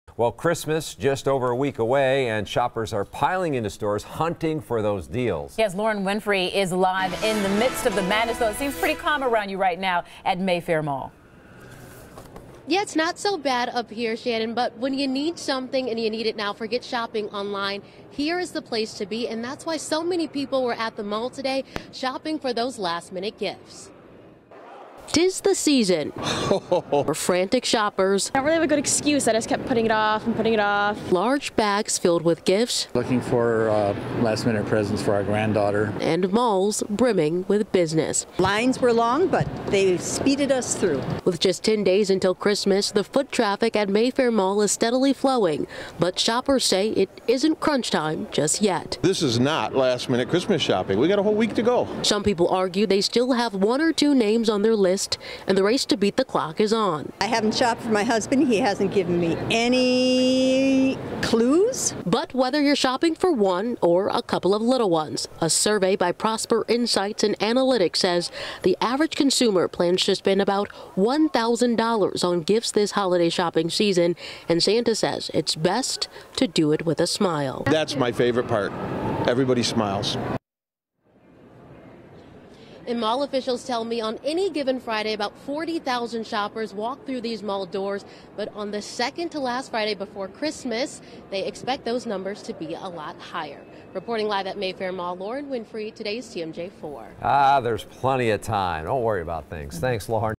Last minute Christmas shoppers hit the malls
Last minute shoppers are packing into stores to get their loved ones their holiday gifts.